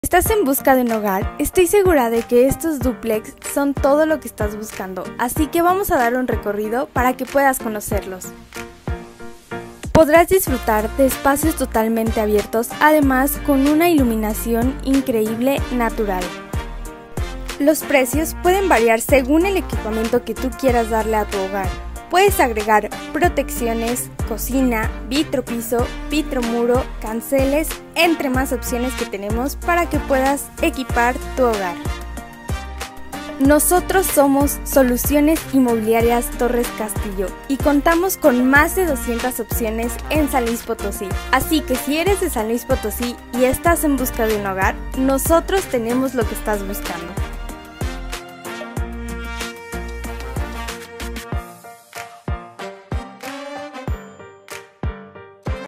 Duplex por Ciudad Satélite desde $500,000 C. 43DUPSA
🤩 🏠¿En Busca de un Hogar? Dúplex en Venta por Carretera a Rio Verde por Ciudad Satélite desde $500,000
Planta Alta: Sala, Comedor, Cocina, 2 Recamaras 🛌, 1 Baño, 1 Cajón de estacionamiento🚘 y Patio de servicio (En Planta + Jardín).
Planta Alta:
$500,000 (Piso Pulido)
$516,000 (Con Vitropiso)
Planta Baja:
$515,000 (Piso Pulido)
$536,000 (Con Vitropiso)
También se puede entregar totalmente equipado, Vitropiso, Cocina, Canceles, Vitromuro en Baño y Cocina y Protecciones
O puedes elegir el equipamiento y cambia el precio.

C. 43DUPSA